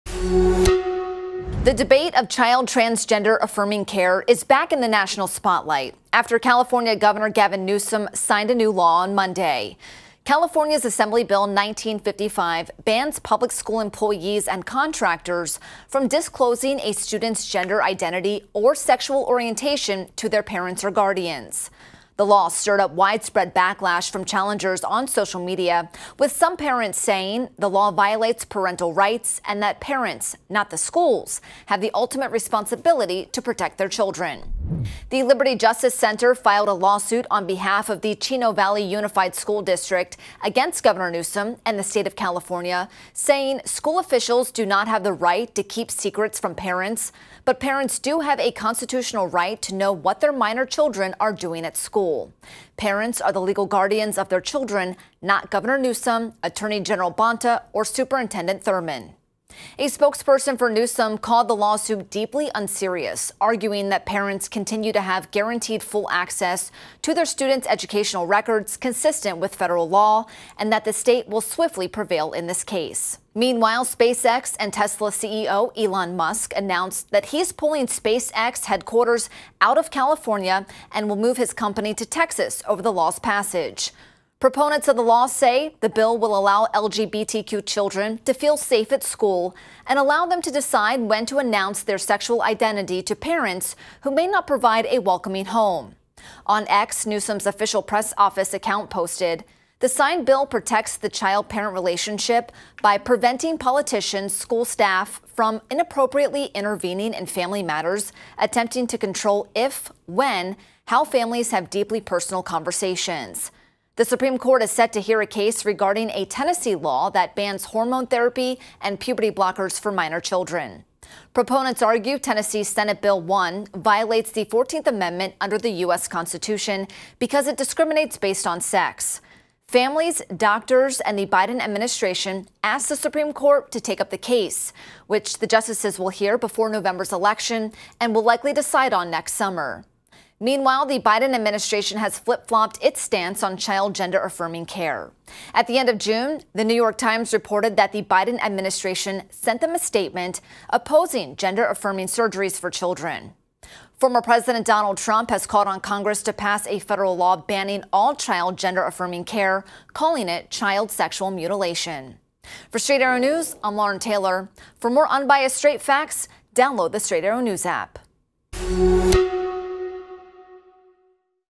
New CA child gender identity law sparks backlash and legal battles
The debate over child transgender-affirming care is back in the national spotlight after California Gov. Gavin Newsom signed a new law Monday, July 15. California Assembly Bill 1955 bans public school employees and contractors from disclosing a student’s gender identity or sexual orientation to their parents or guardians.